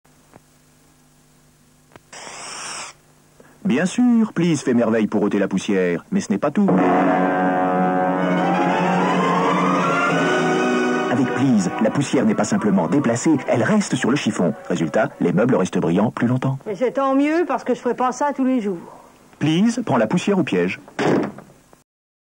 [Pliz] Madame Chiffon, 1981
Quand elle tourne ce spot au tout début des années 80, Marie-Pierre Casey est loin de se douter de l'immense notoriété qu'elle en tirera, en partie grâce à sa fameuse réplique « Et c'est tant mieux parce que je f'rai pas ça tous les jours… ». Elle reçut le prix Minerve de la meilleure actrice dans une publicité télévisée.

[Pliz] Madame Chiffon. Agence : DDB Communication France. Réalisateur : Dominique Cheminal. Production : Franco. American Films. Date : 1981. Film couleur/sonore. Durée : 20 secondes.